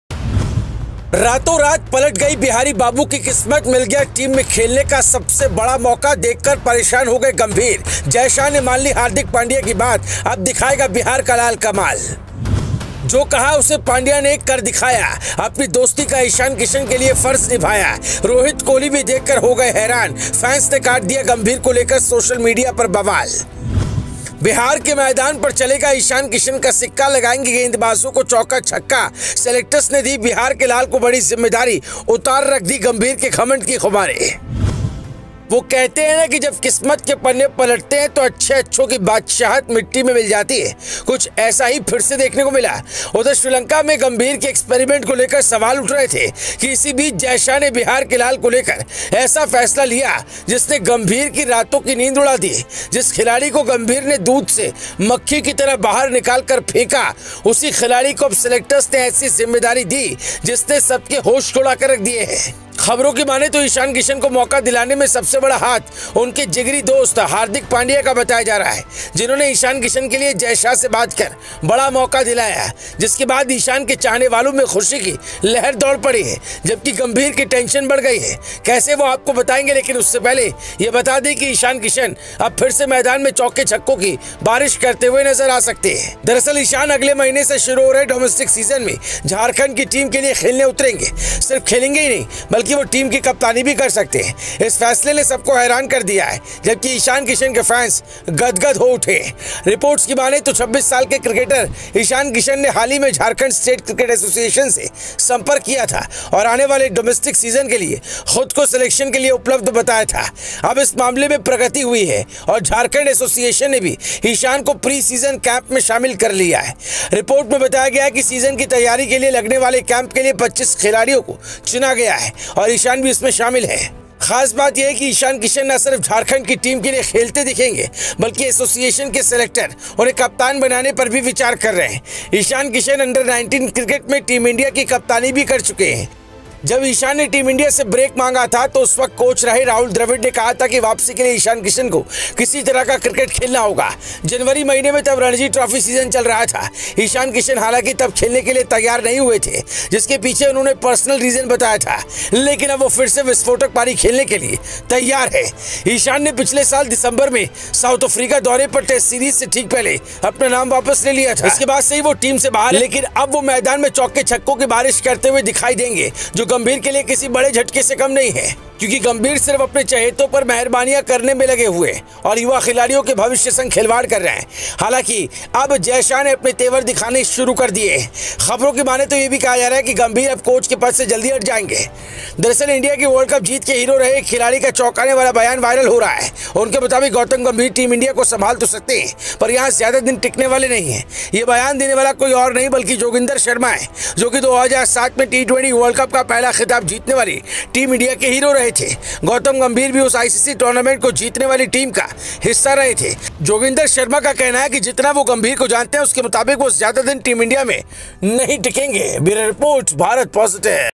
रातों-रात पलट गई 'बिहारी बाबू' की किस्मत, मिल गया सबसे बड़ा मौका, देखकर परेशान हो गए Gambhir
रातों-रात पलट गई 'बिहारी बाबू' की किस्मत, मिल गया टीम में खेलने का सबसे बड़ा मौका, देखकर परेशान हो गए Gautam Gambhir